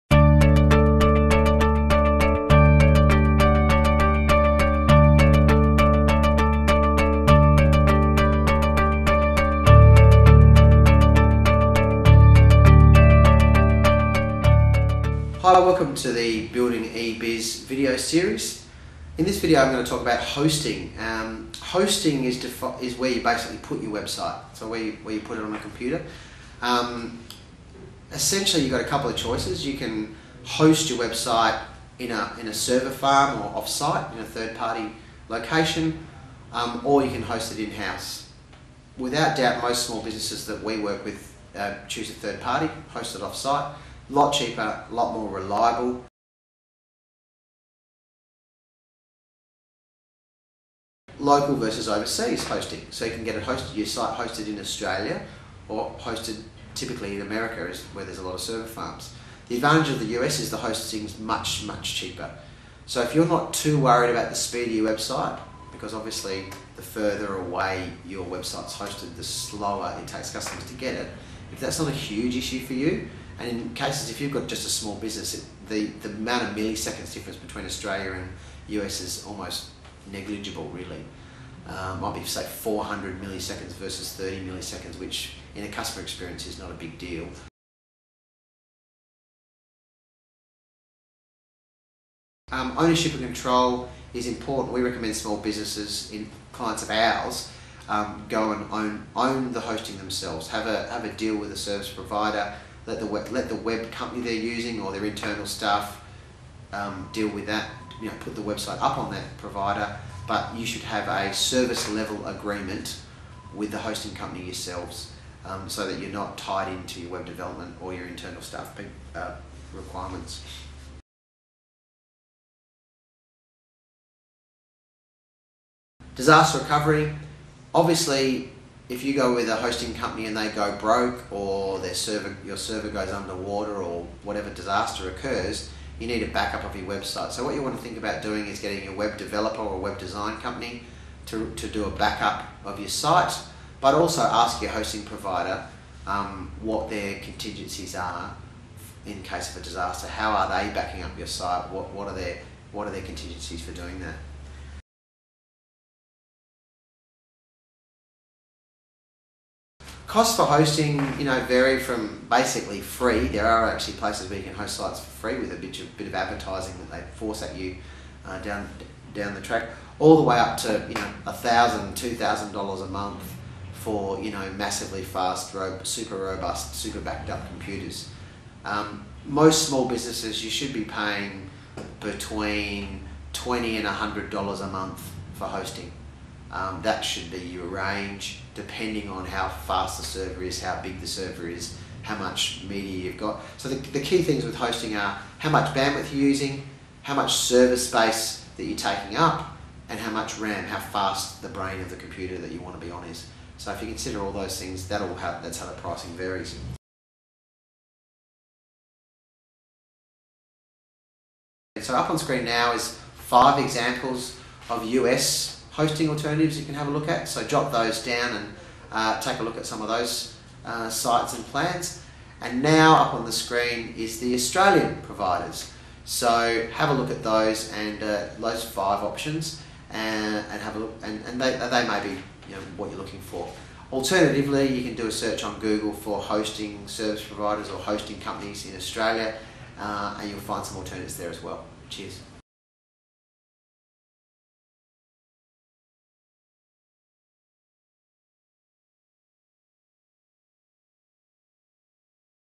Website Hosting Explained; Understanding web hosting - Building eBusiness
Watch this video to gain an insight into what a website host is, what the best website hosting options are for your small business, and where to purchase them.